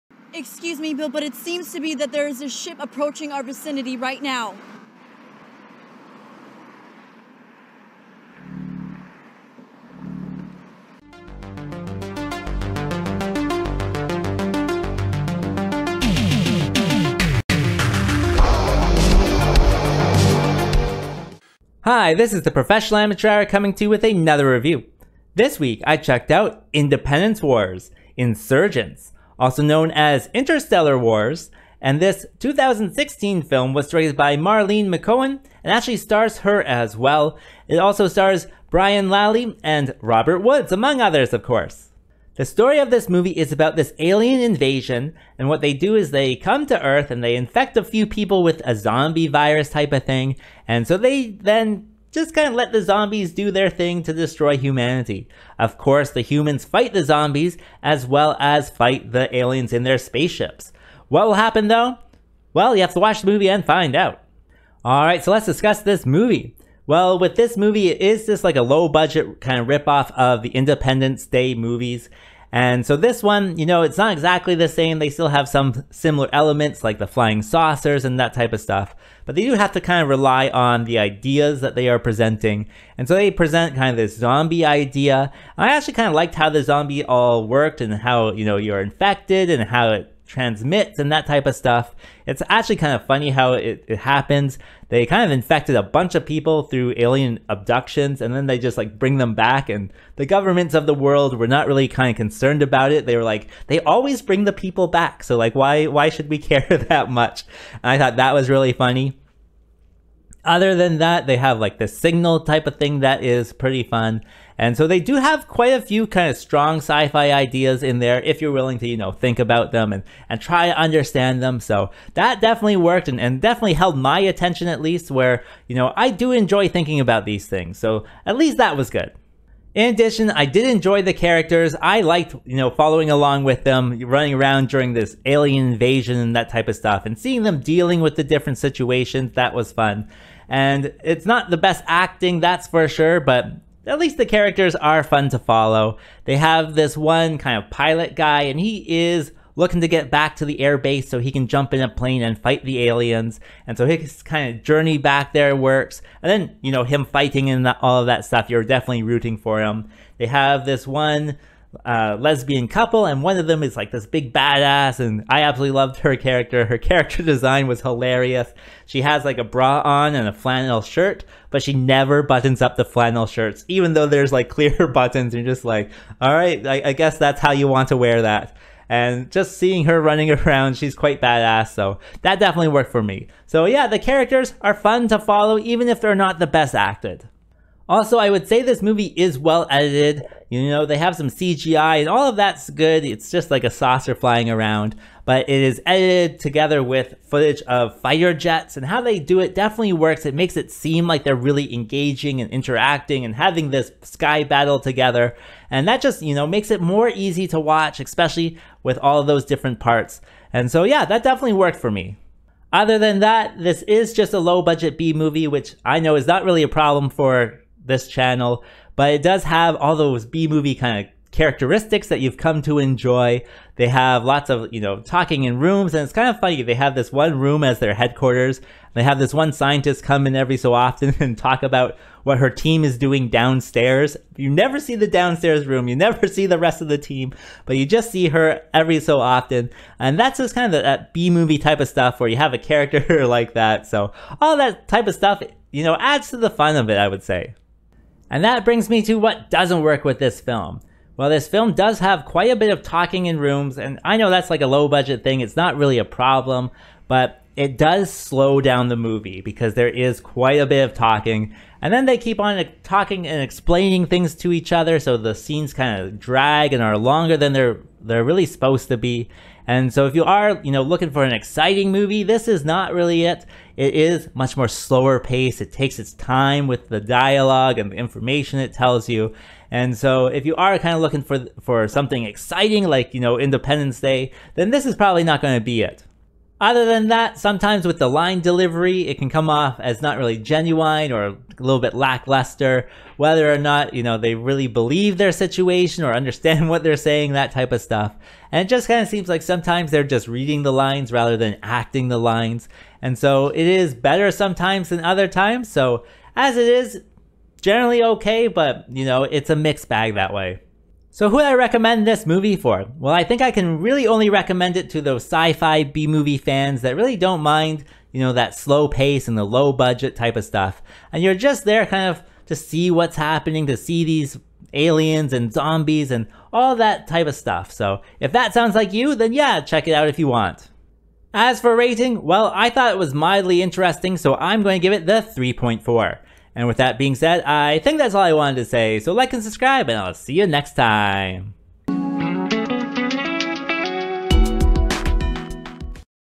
INTERSTELLAR WARS ( 2016 Marlene Mc'Cohen ) aka INDEPENDENCE WARS : INSURGENCE Sci-Fi Movie Review
From a giant portal behind the dark side of the moon aliens starts an attack on earth. From a secret underground base scientists try to find a way to fight the aliens. Time is of the essence as people are dying and soon the aliens have full control.